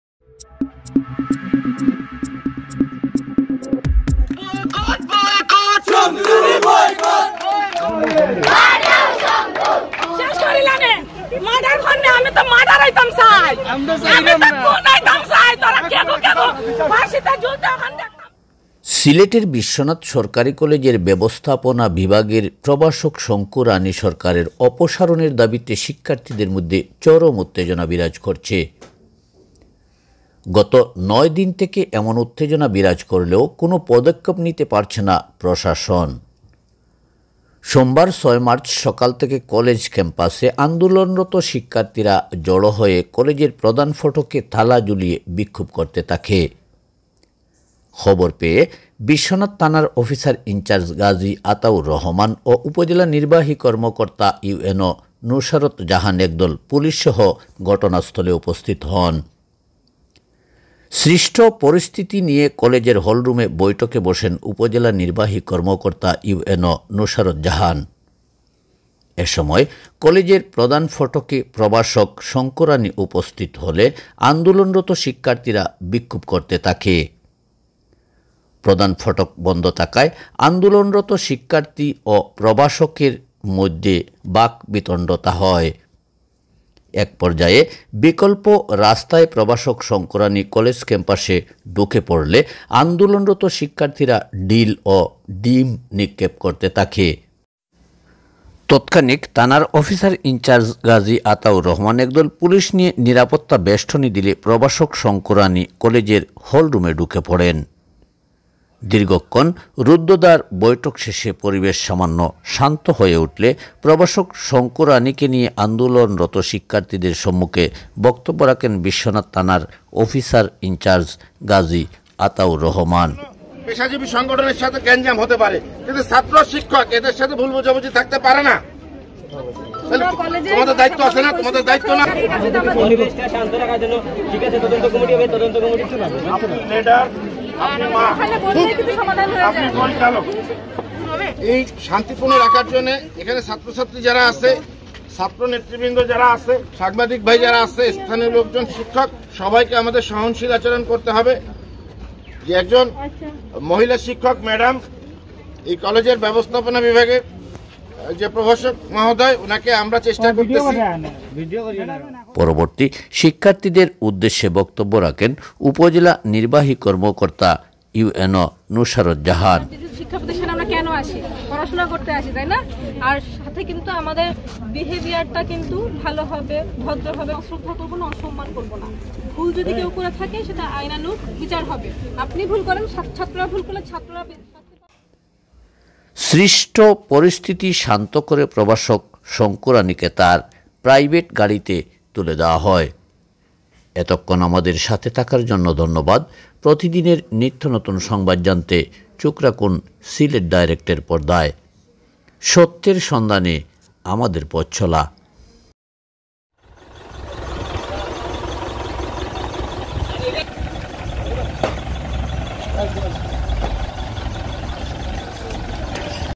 বিশ্বনাথ কলেজের প্রভাষক শংকু রাণীর অপসারণের দাবীতে চরম উ-ত্তে-জ- না
সিলেটের বিশ্বনাথ সরকারী কলেজের ব্যাবস্হাপনা বিভাগের প্রভাষক শংকু রাণী সরকারের অপসারণের দাবীতে শিক্ষর্থীদের মধ্যে চরম উ-ত্তে- -জ-না...